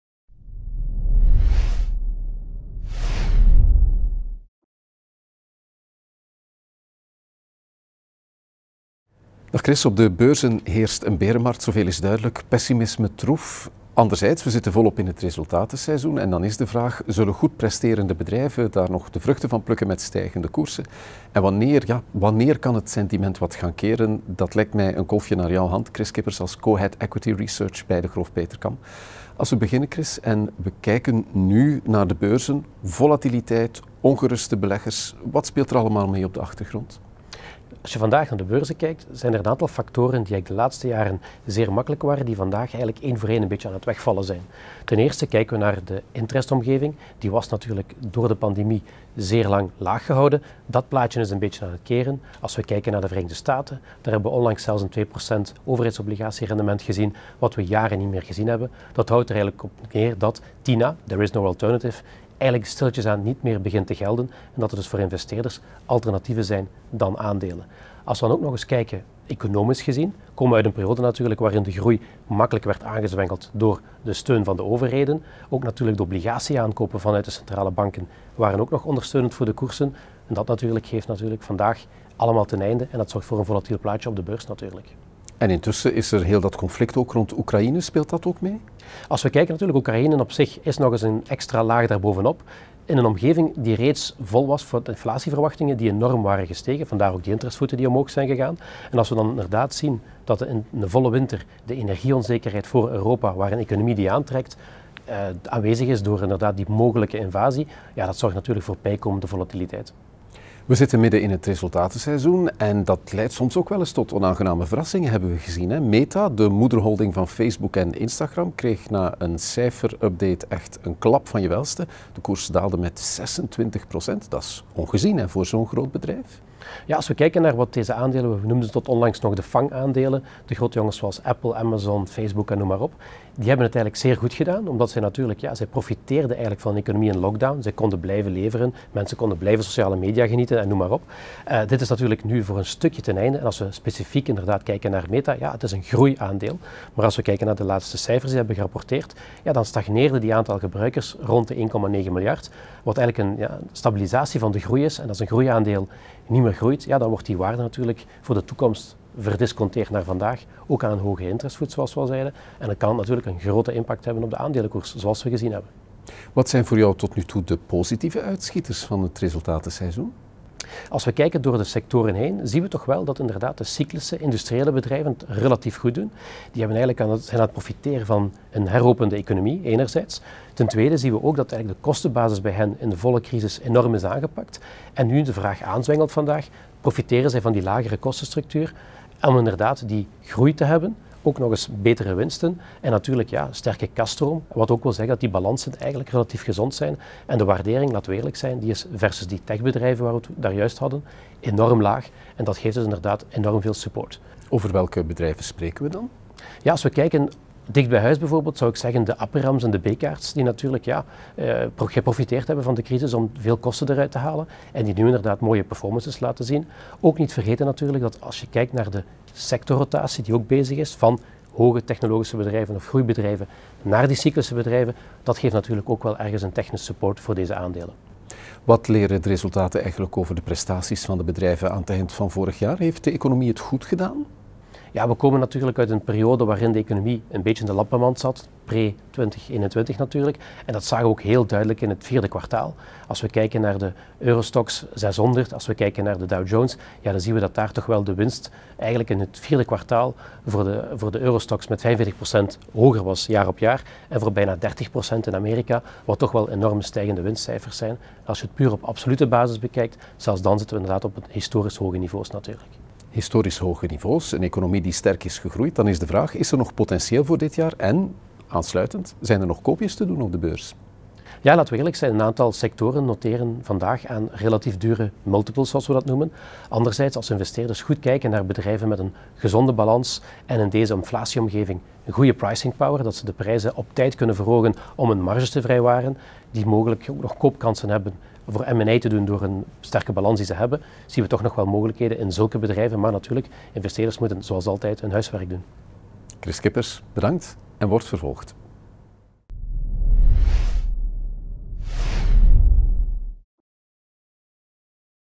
Inflatie, Oekraïne, volatiliteit: zijn er nog koopjes op de beurs?
Op de aandelenbeurzen heerst sinds kort een berenmarkt. Pessimisme lijkt te overheersen, terwijl we toch midden in het resultatenseizoen zitten. Zullen goed presterende bedrijven nog de vruchten plukken met een stijgende koers? En wanneer kan het sentiment keren? Kris Kippers, co-head Equity Research bij Degroof-Petercam, geeft zijn analyse.